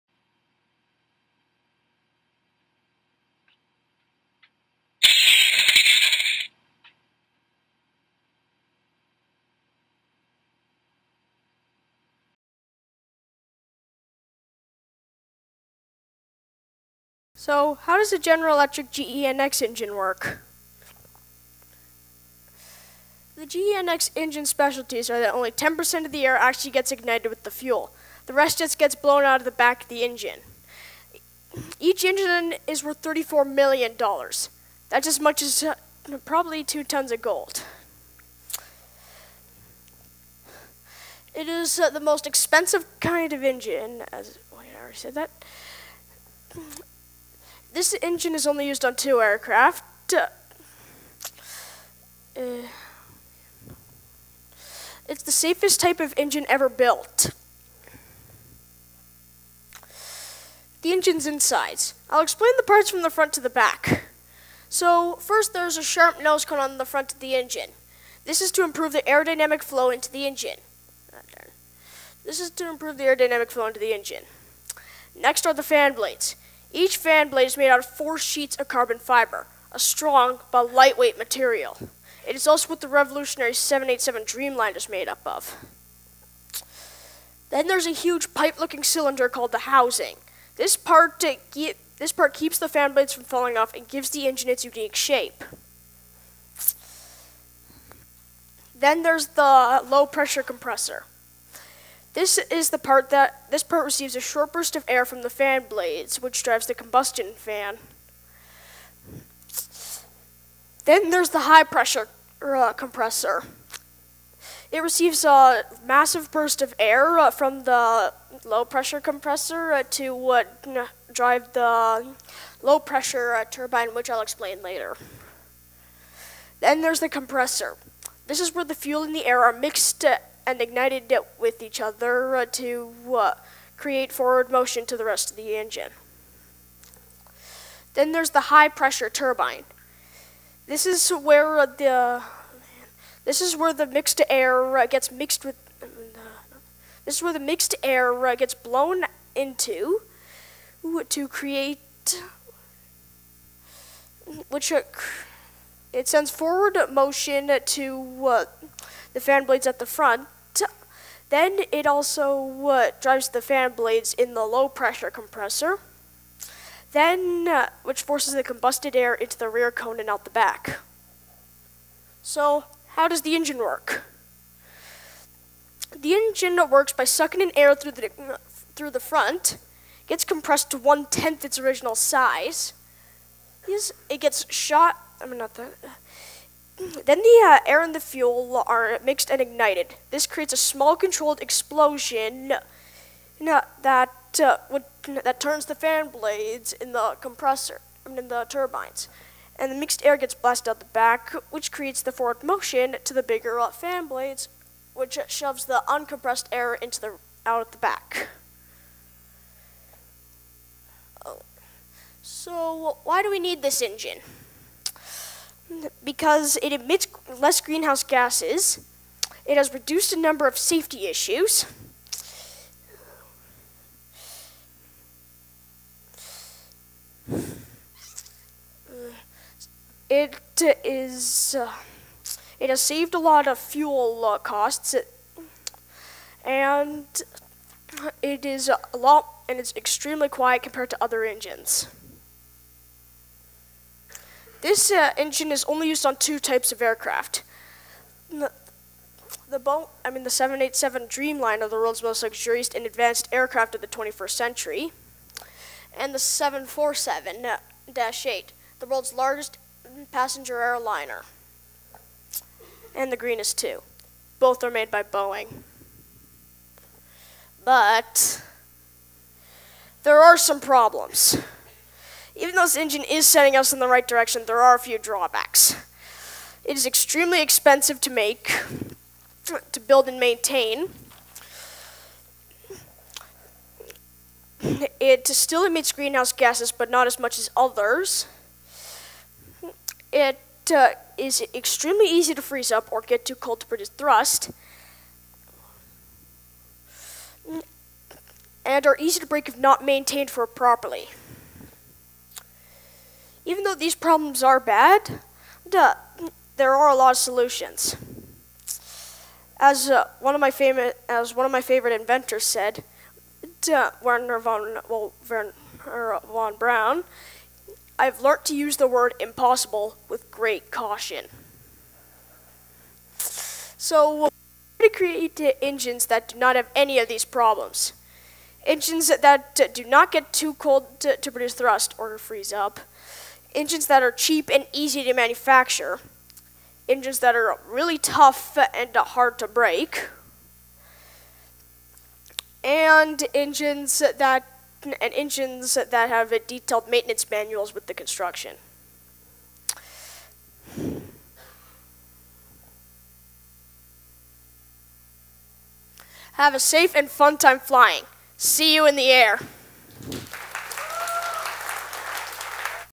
The Best Engine of All Time | Mathew Dixon | TEDxRundleAcademy
General Electric's GENX engine is a feat of engineering prowess. In this short, charming talk, young Mathew Dixon explains why this engine is remarkable. However, it is his musings upon the importance of innovation that make this talk captivating.

Mathew Dixon is a grade 6 student who is fascinated by innovation, engineering and airplanes. He wants us to all know that impossible is not in his vocabulary.